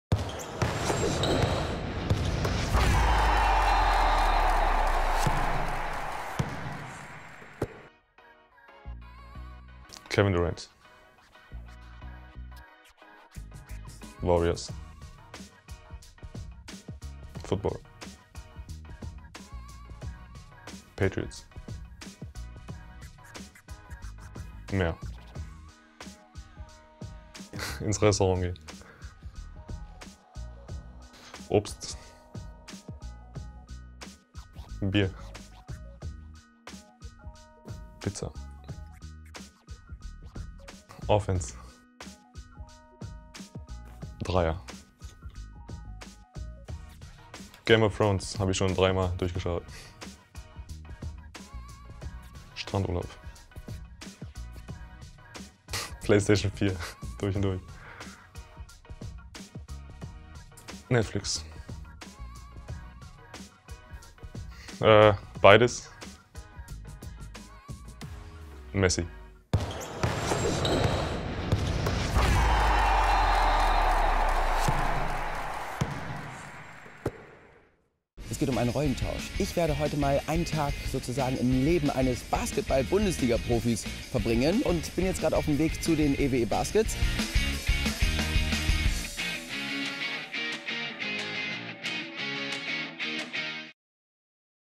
Gelb oder Blau - Folge 7: Haris Hujic
Entweder... oder...? Es geht um Film, Musik, Essen, andere Sportarten und mehr: Was mögen die Stars der EWE Baskets Oldenburg? Mit unserer Video-Serie "Gelb oder Blau" wollen wir der Sache auf den Grund gehen. In der 7. Folge stellt sich Haris Hujic dem Fragen-Stakkato. Alle bisherigen Folgen der Serie sowie weitere Videos gibt es auf unserer